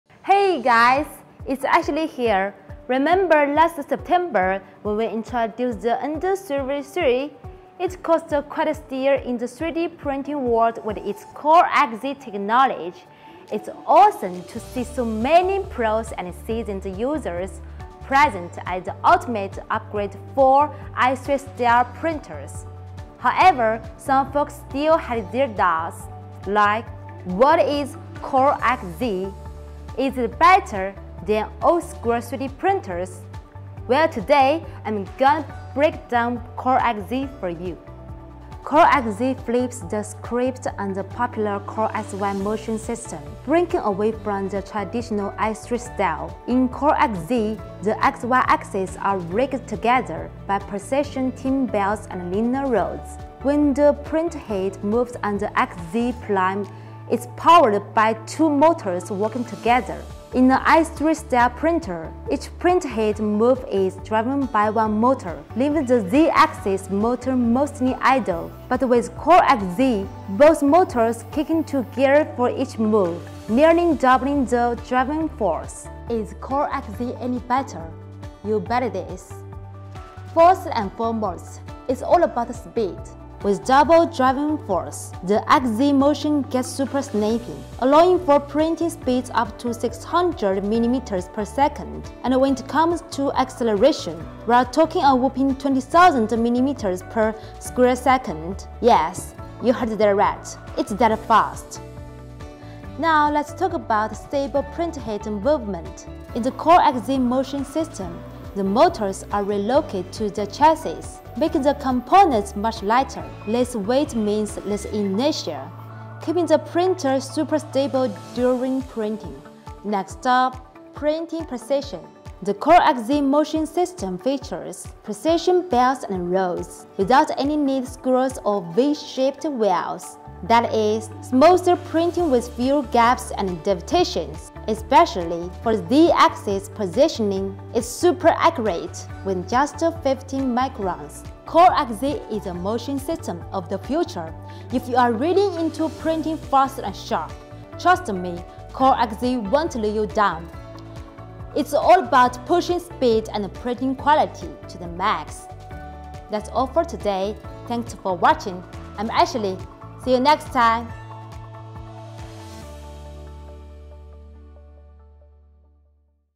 What does CoreXZ Motion System Mean for a 3D Printer?
What does CoreXZ mean? Will it outperform other CoreXY printers? Why have we adopted the CoreXZ motion system for our two flagship printers - Ender 3 V3 and the larger Ender 3 V3 Plus? All the answers to these questions can be found in this video. Check it out if you're interested!

The V3 series flagship, the larger 300X300X300 printer - Ender 3 V3 Plus, will start selling on May 8th. Set your alarm so you won't miss it!